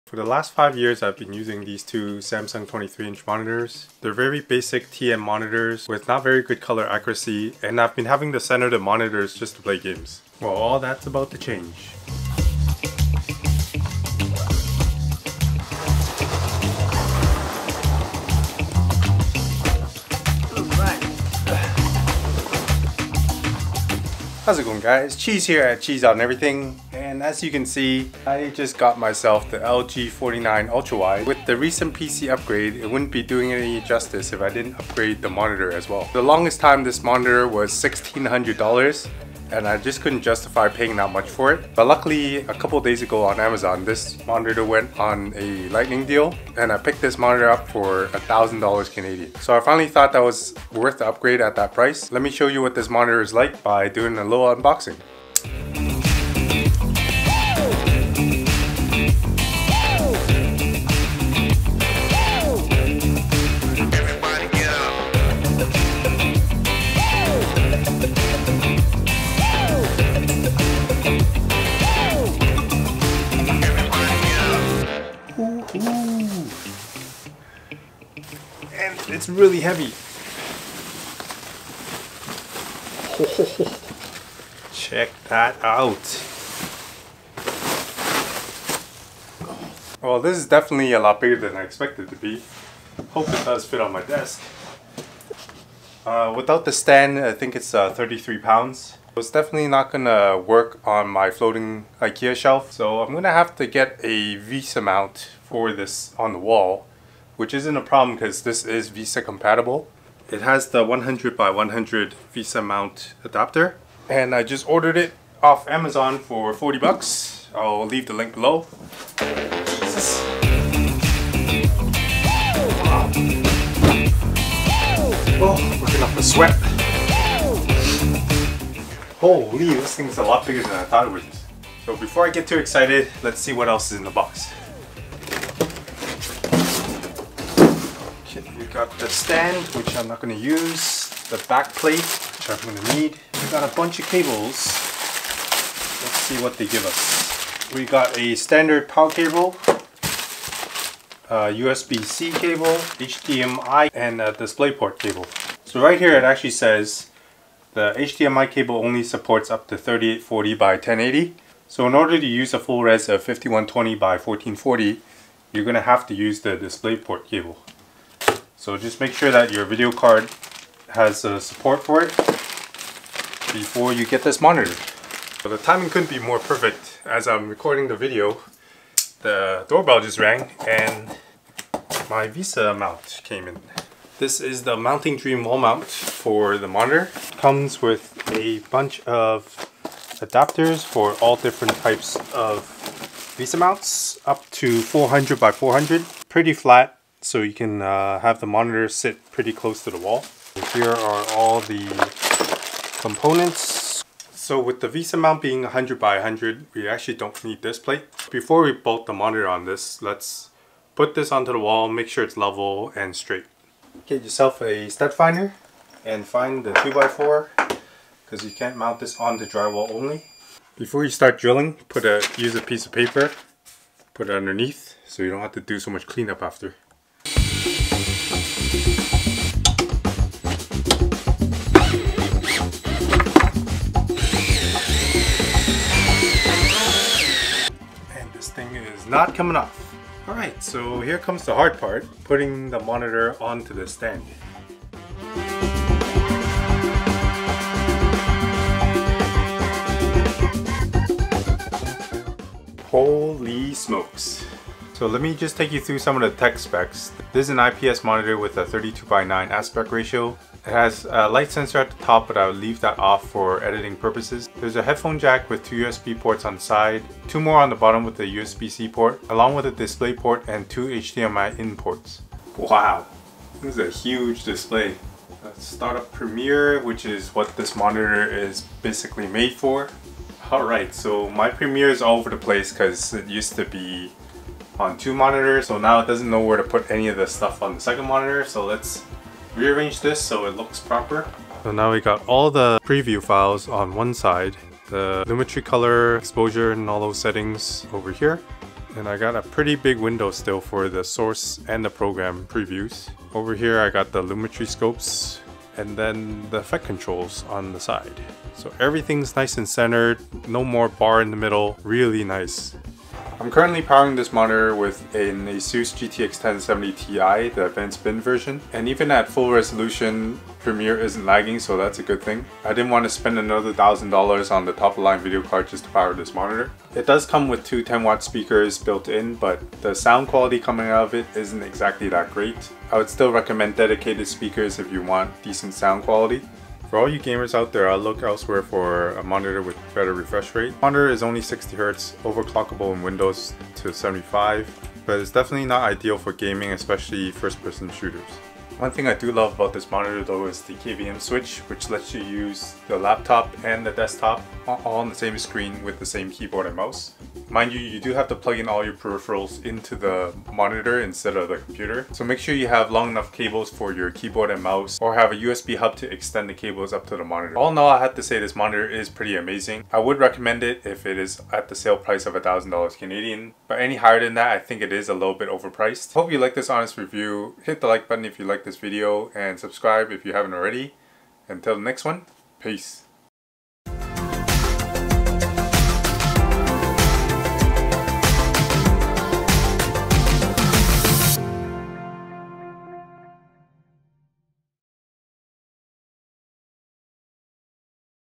LG 49" Super Ultrawide 32:9 5120x1440 display 49WL95C-W (bought, not sponsored)
Amazon Canada:
LG 49' Super Ultrawide monitor
Mounting Dream wall mount
Dewalt Drill set

Amazon US:
Dewalt Drill set

The LG 49 inch ultrawide monitor is a creator's dream.  Having the Adobe Premiere timeline stretch the length of two 27 inch monitors is such a great tool to work with.